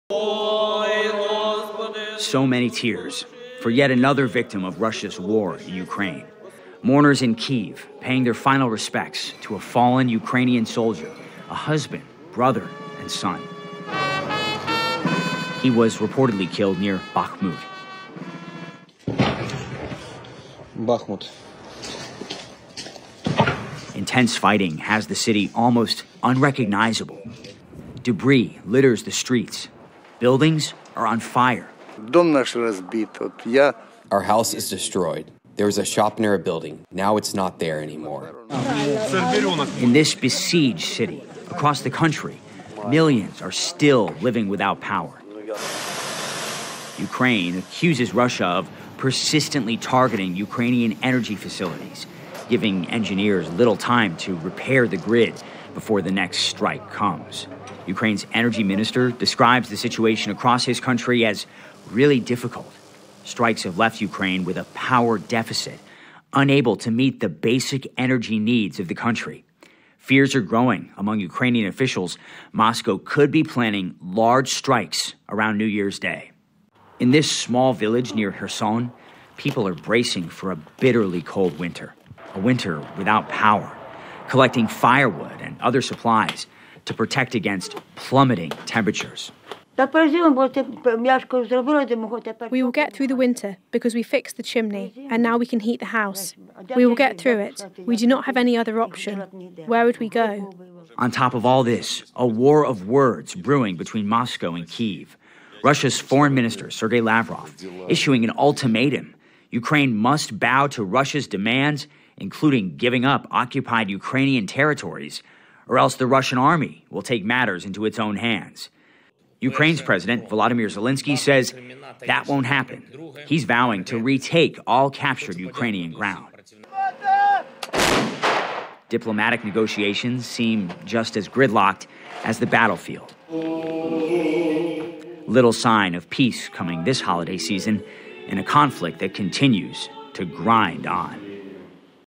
Russian foreign minister: Ukraine must give up occupied territories
Russia's Foreign Minister Sergey Lavrov issued an ultimatum to Ukraine advising the country to agree to Russia's demands, which includes giving up occupied territory within Ukraine. CNN's Will Ripley reports.